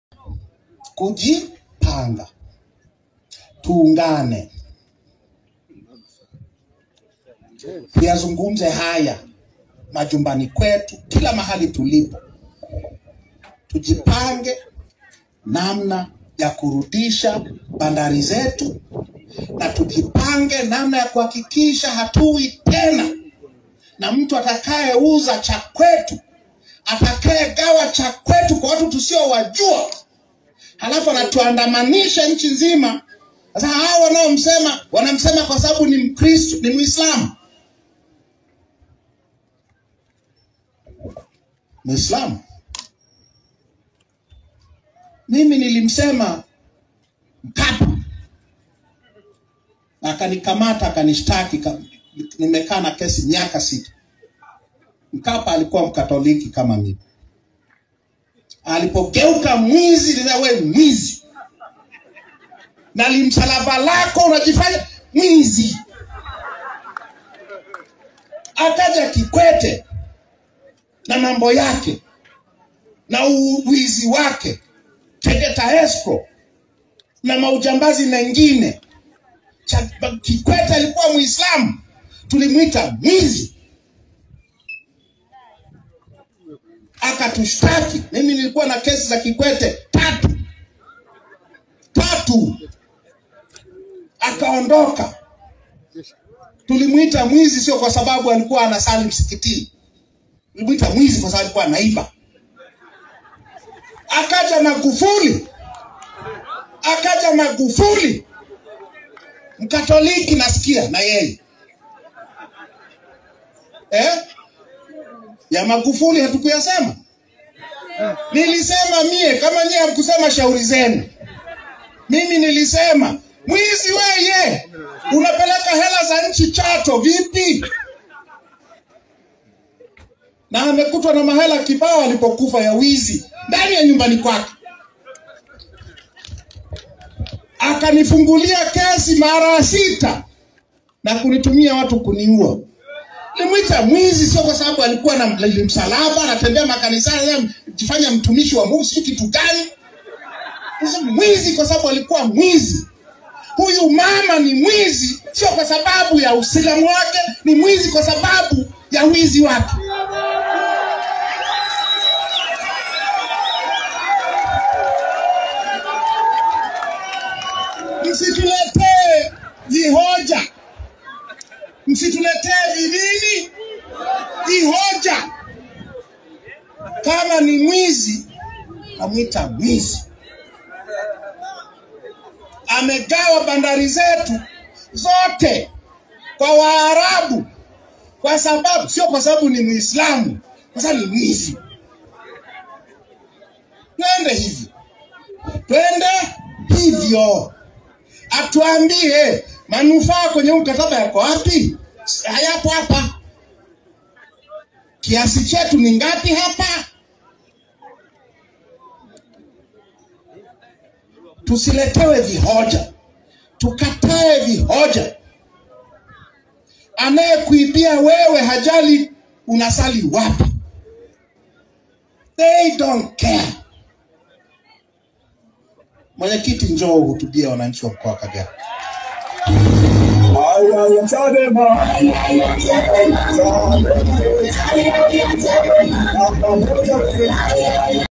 TUNDULISU AFUNGUKA MARAIS ALIOWAHI KUWAITA WAIZI NA KUFUNGULIWA KESI "NI KWELI NI WAIZI SIO UONGO.."
WeAreEverywhere